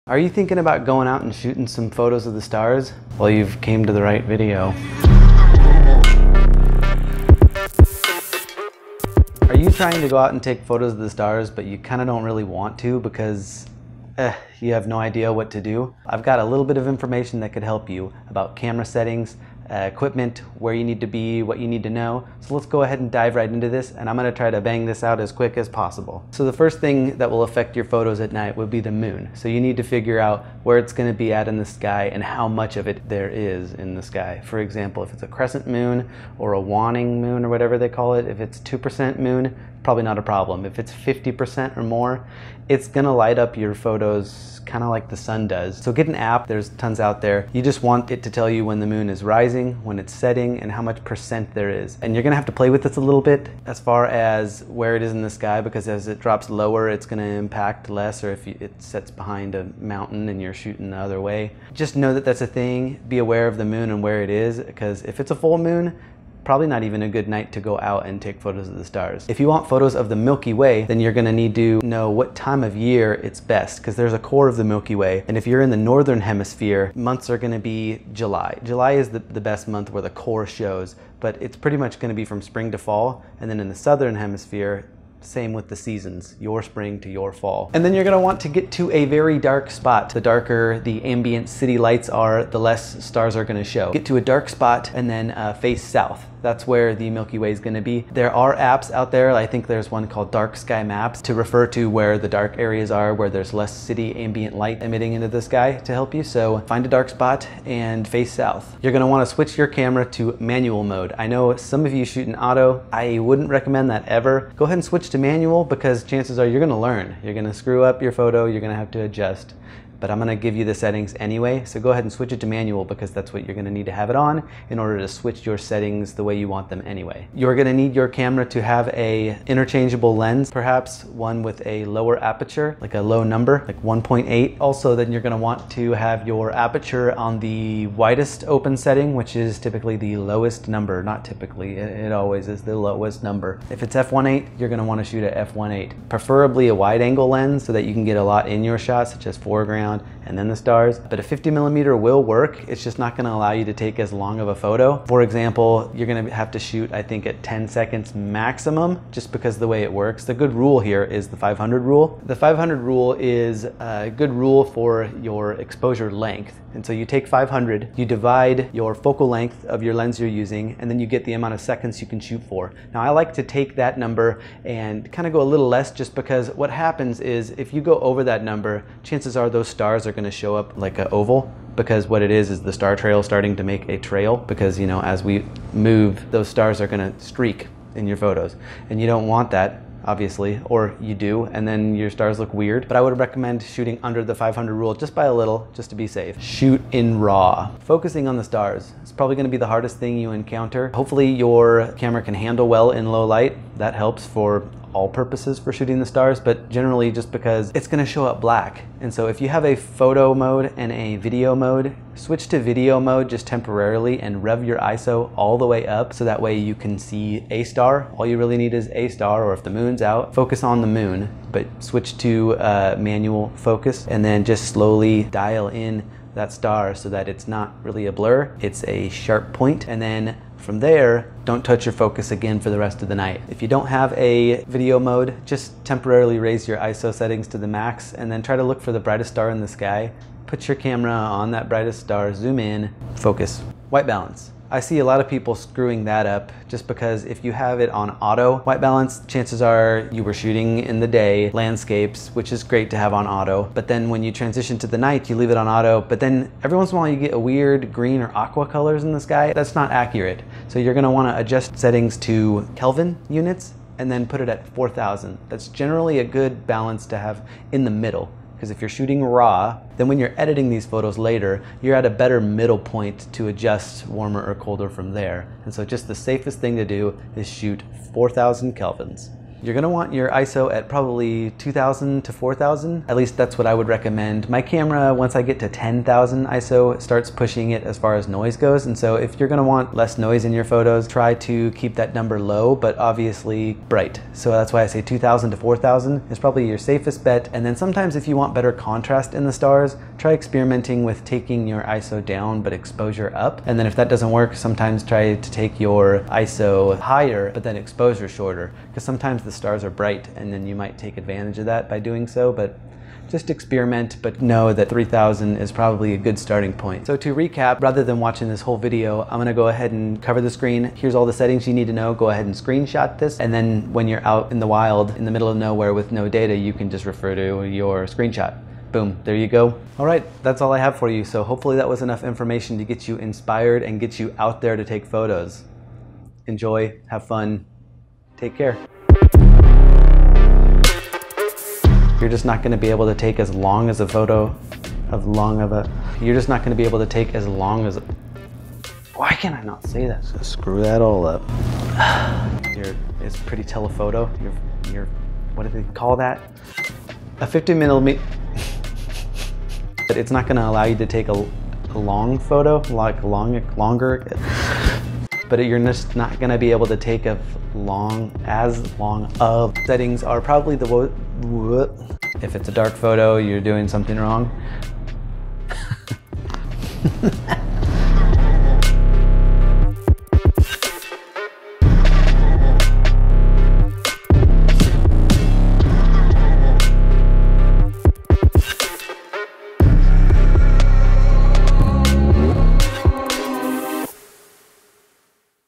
HOW TO SHOOT THE STARS (Easy beginner guide)
How to take photos of the stars, the easiest way.

I don't normally make instructional videos but I figured I would try one. This is what I needed when I started shooting astrophotography so I thought I would be of help to whoever stumbles across this. If you think I should do more about different topics, let me know!

I hope you enjoy the video. Also hope this inspires you to channel your inner child and get outside tonight and have some fun!

| Filmed using
Nikon Z6
Nikon Zoom Lens
Nikon Portrait Lens
Manfrotto Bag
Manfrotto Tripod
Lav Mic
Mic Recorder